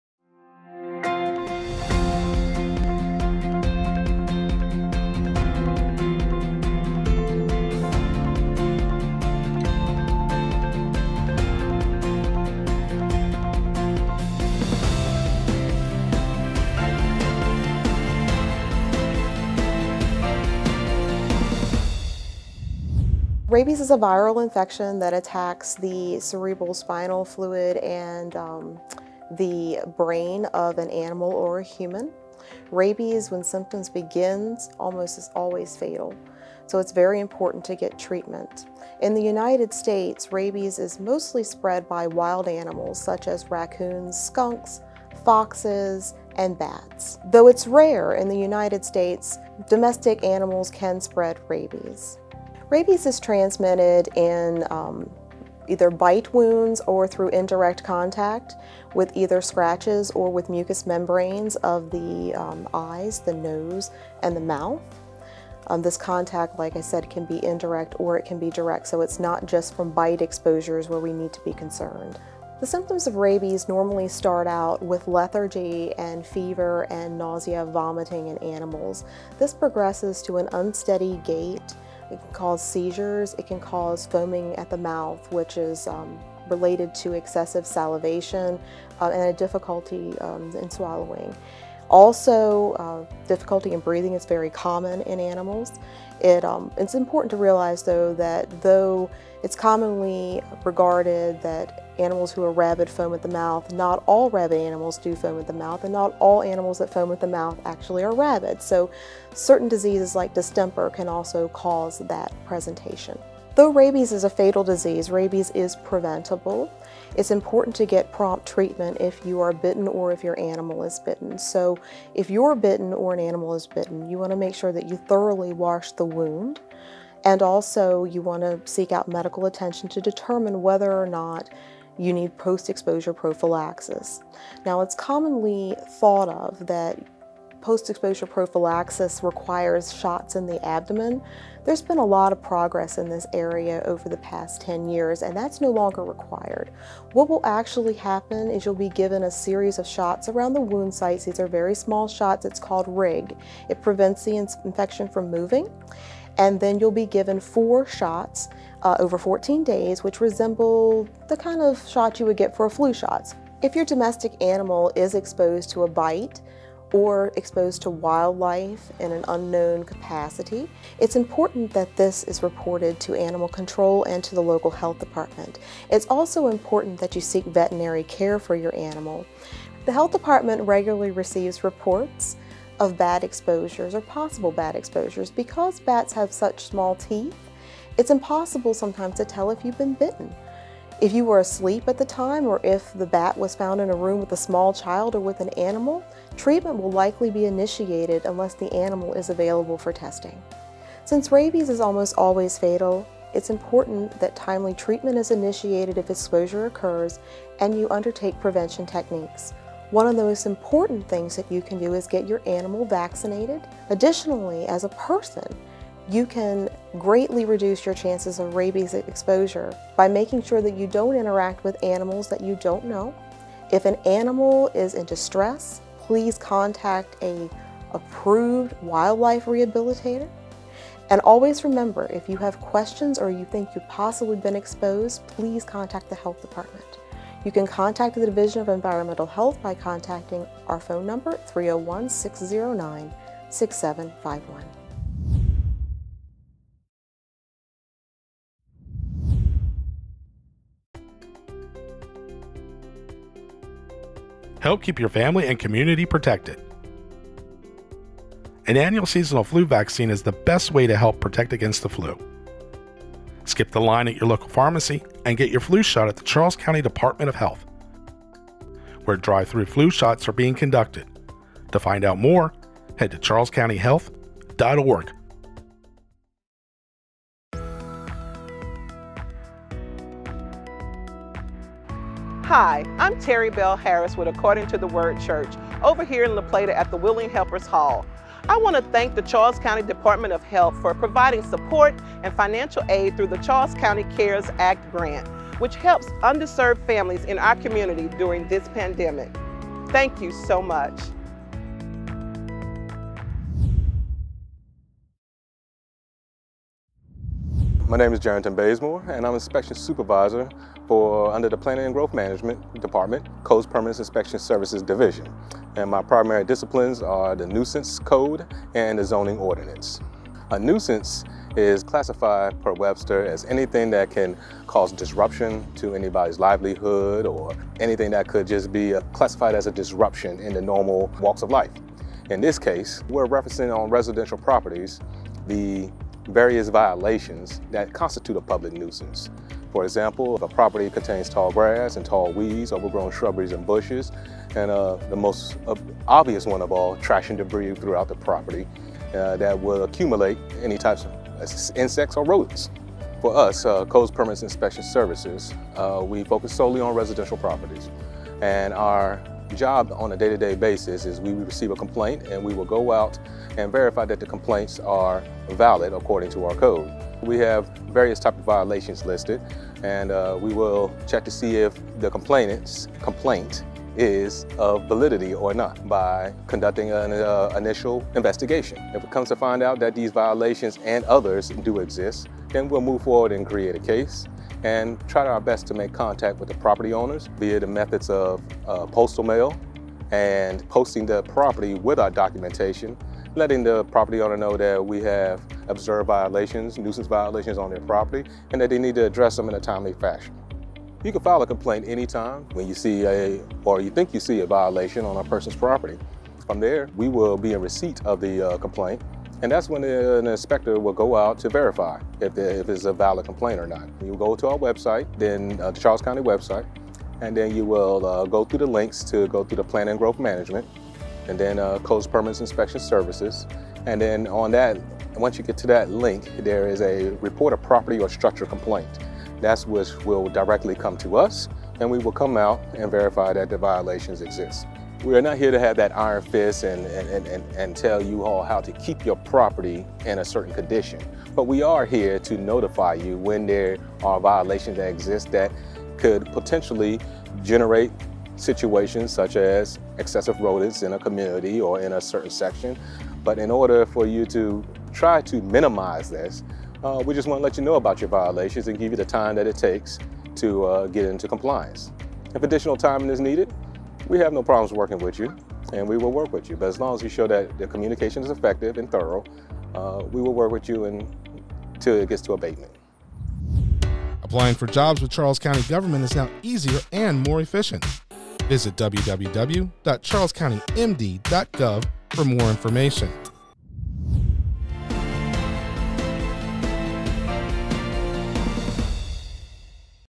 Your Charles County November 23, 2020
This week on Your Charles County, Lisa Laschalt, the Director of Enviromental Health, shares important information on rabies. Also, Jarington Bazemore, Inspections Supervisor for Planning and Growth Management, gives details on nuisances and what to do about them.